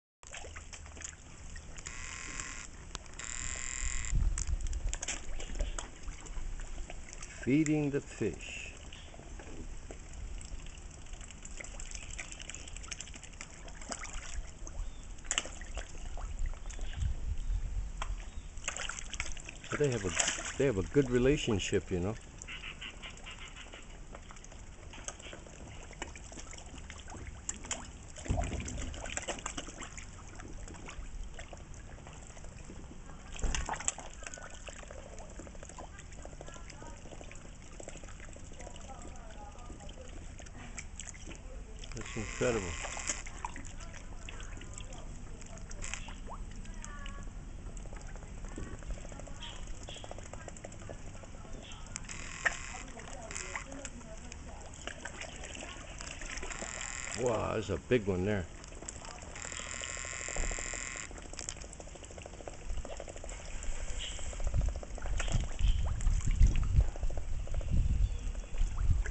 Black swan feeding Koi fish
L'abnégation est un renoncement partiel ou complet de soi, et de ses intérêts personnels pour servir autre chose, dans le soucis du bien être d’autrui,belle leçon du monde animal...!